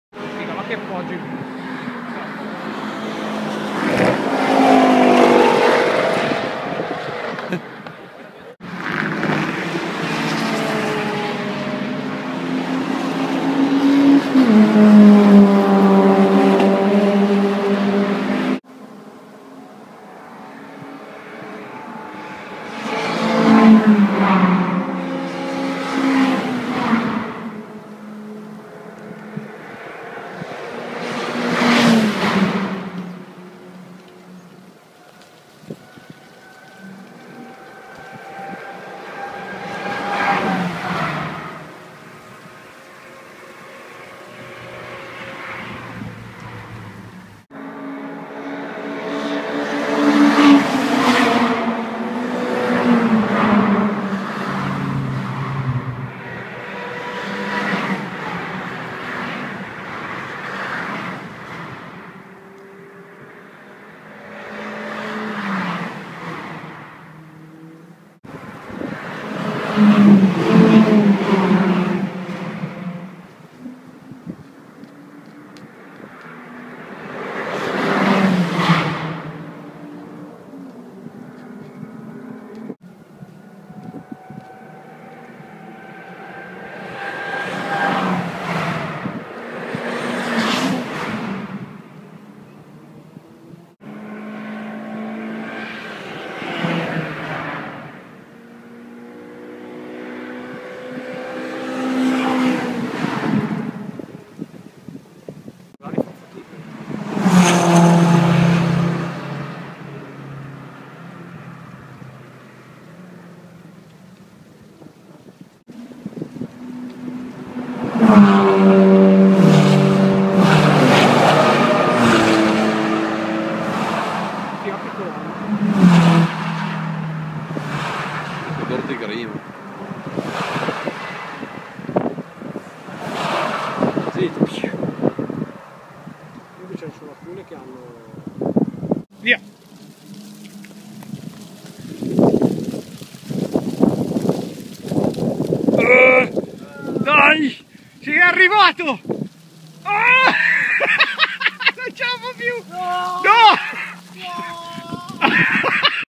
Monza Speed Day 03/02/2013 - Track Day Autodromo
Vari passaggi speed day del 3 Febbraio 2013 a Monza e io sulla parabolica Nord che quasi esco dalle scarpe